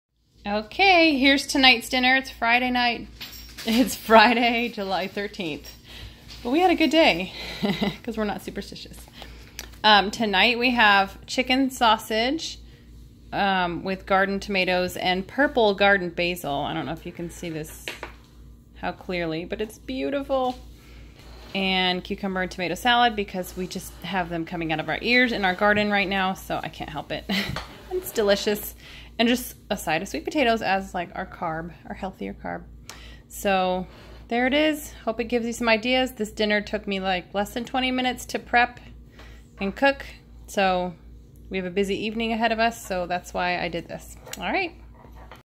Dinner-Friday, July 13, 2018-chicken apple sausage with veggies
Organic chicken apple sausage sautéed with garden tomatoes and purple garden basil, cucumber tomato salad and organic sweet potatoes.
For dessert I made a Wheat Belly (wheat free) cookbook brownie recipe without sugar (monk fruit/erythritol blend and coconut sugar to make it sweet).